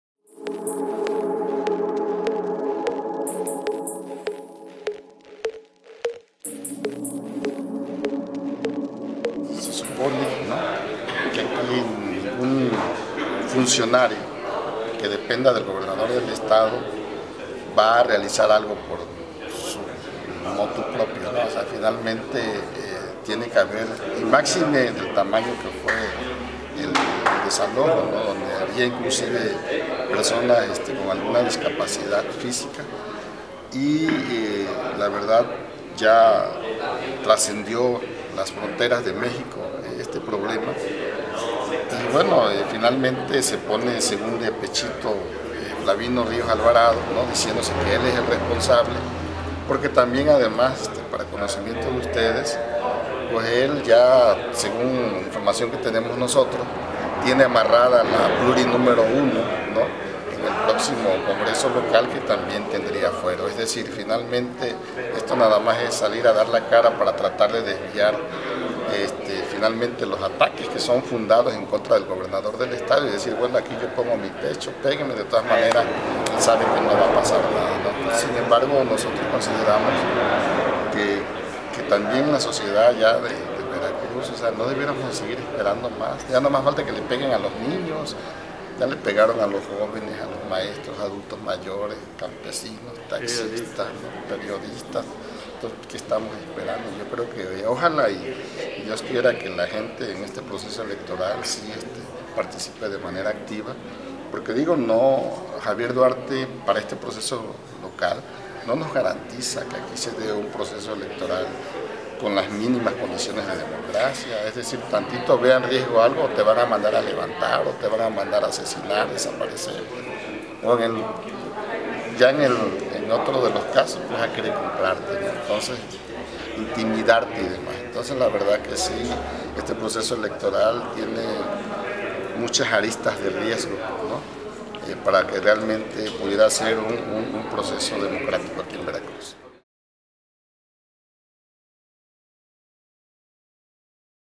Flavino dio la cara porque tiene amarrada la plurinominal: PT
Xalapa, Ver.- Luego de la represión que sufrieron jubilados y pensionados por parte de la Secretaría de Seguridad Pública, el secretario de gobierno, Flavino Ríos, salió a aceptar la responsabilidad, pero integrantes del Partido del trabajo aseguran que es porque ya tiene amarrada una diputación por la vía plurinominal.